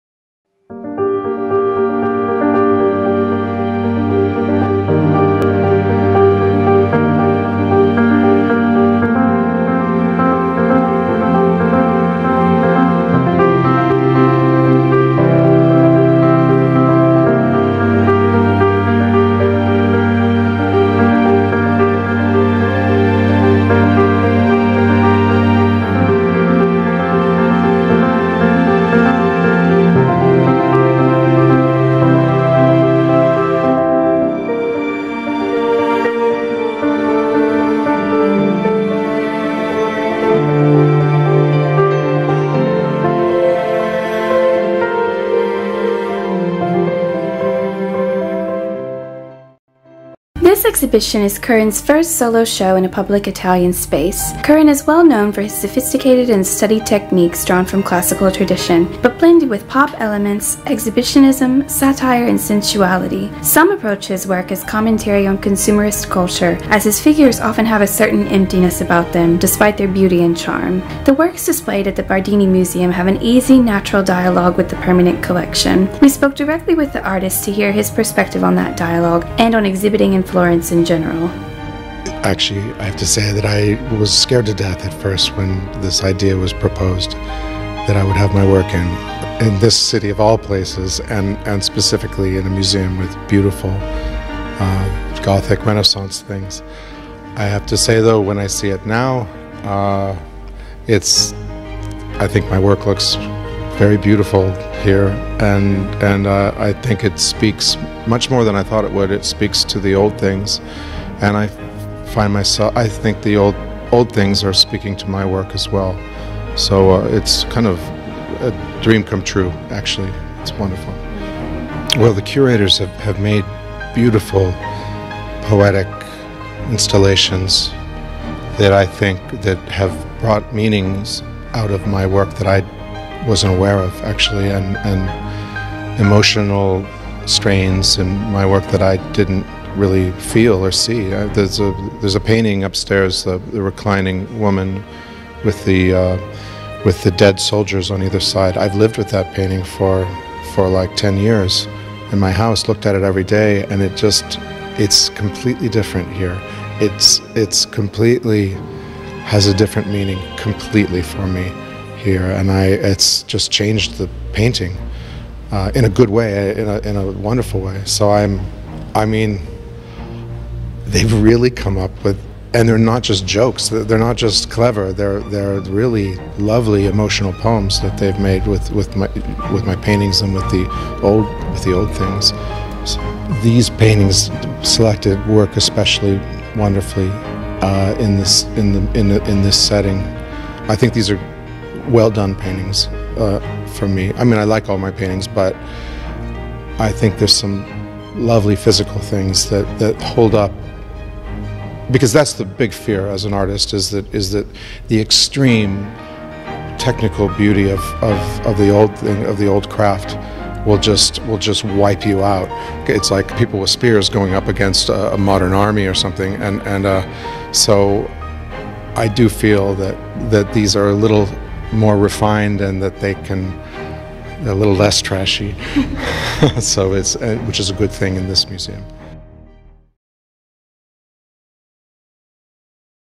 John Currin in Florence
Джон Керрин
Групповая выставка ‘Naturally Naked’ в Галерее Гари Татинцяна.
15 Августа — 15 декабря 2019
Вход свободный. 18+
Group show ‘Naturally Naked’ at Gary Tatintsian Gallery, Moscow.
August 15 — December 15, 2019

John Currin: Paintings
Courtesy Mus.e Firenze, Gagosian gallery and the artist.
Video by Bunker Film.

Courtesy LdM NEWS.